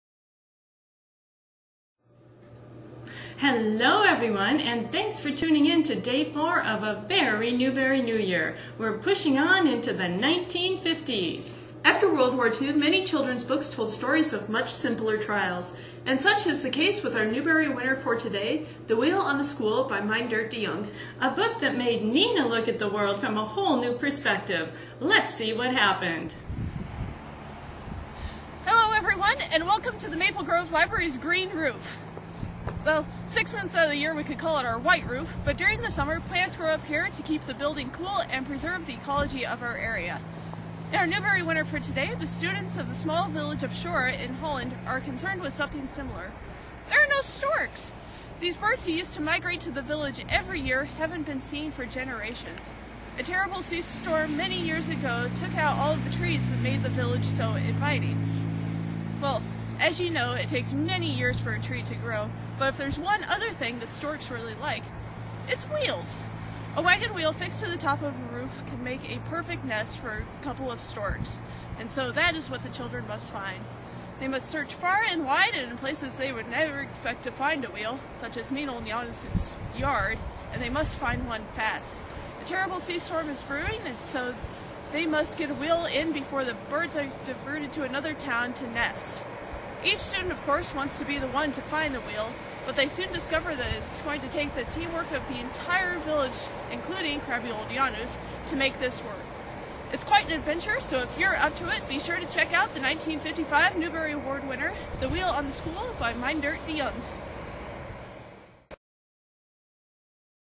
A Very Newbery New Year Day 4: The 1950s
Ann and Nina, Maple Grove Librarians, celebrate Day 4 of A Very Newbery New Year with their book talk of the 1955 Medal Winner, The Wheel on the School by Meindert DeJong.